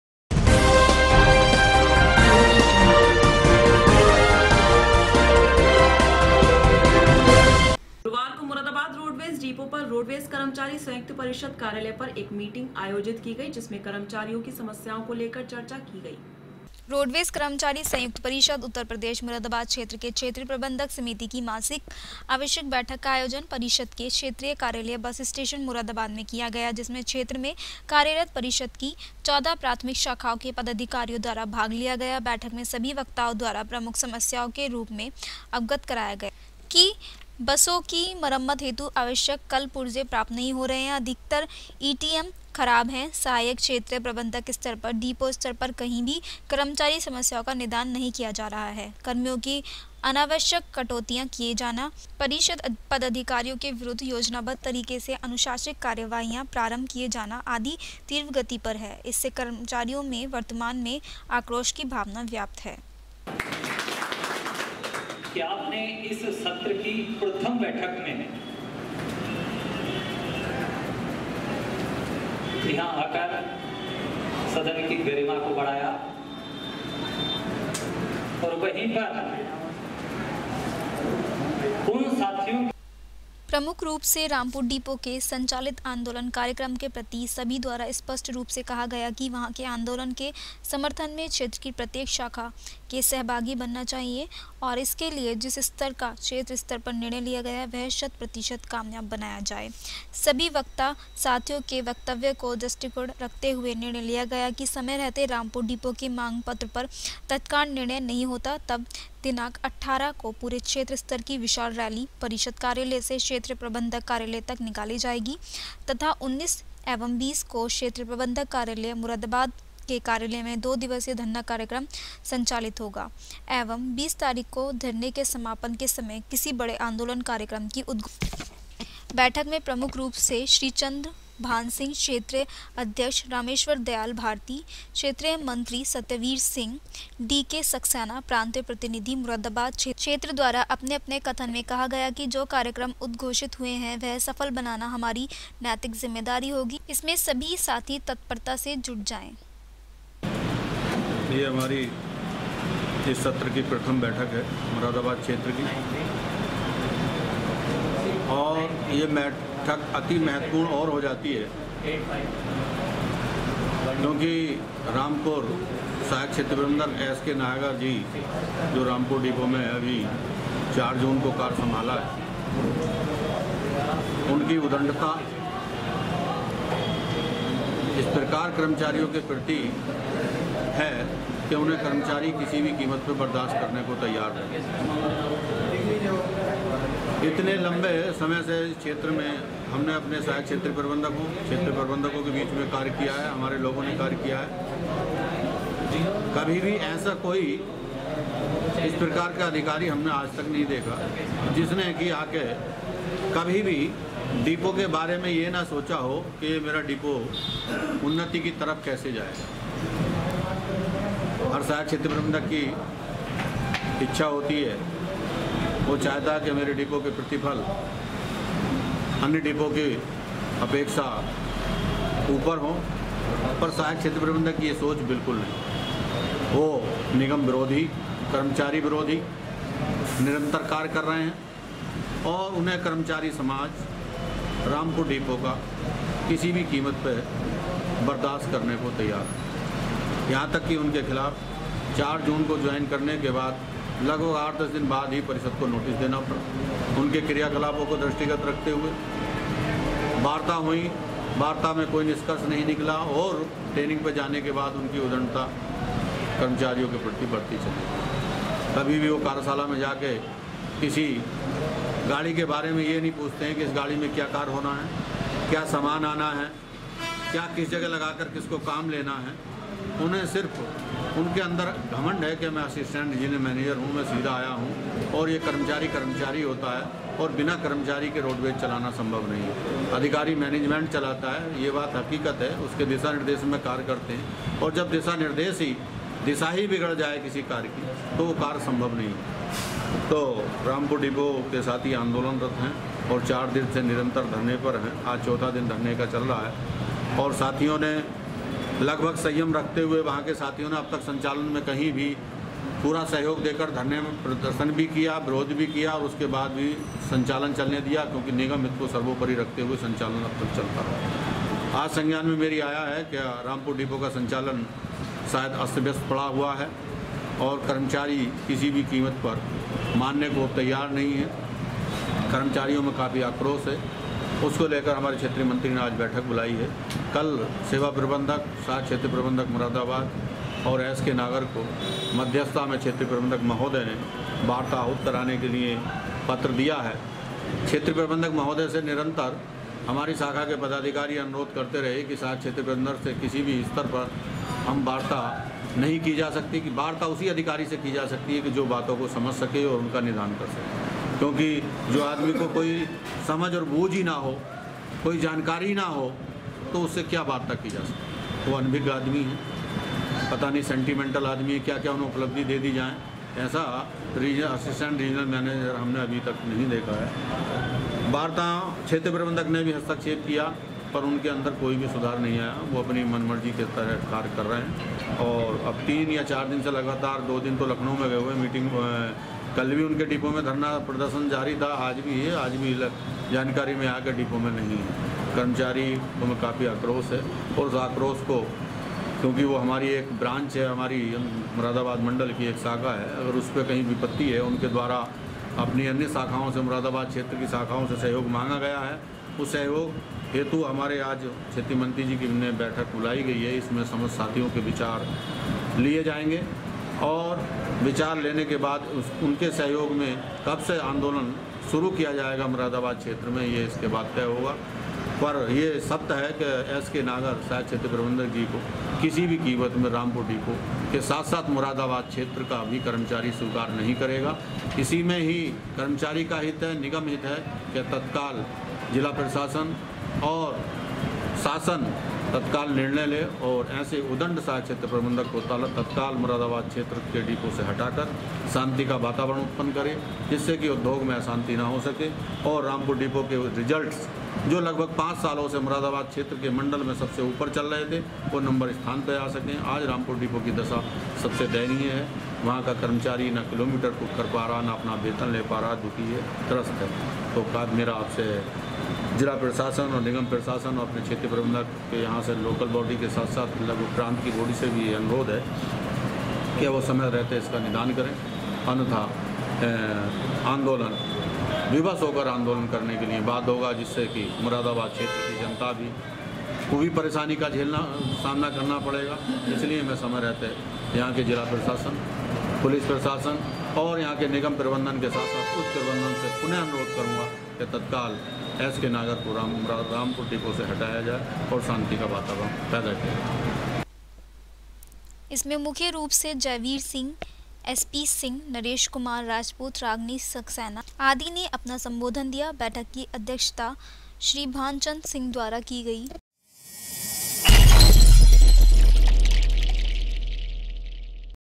ROADWAYS कर्मचारी की समस्याओं को लेकर मीटिंग आयोजित II SS NEWS II MORADABAD
गुरुवार को मुरादाबाद रोडवेज डिपो पर रोडवेज कर्मचारी संयुक्त परिषद कार्यालय पर एक मीटिंग आयोजित की गई जिसमें कर्मचारी की समस्याओं को लेकर चर्चा की गई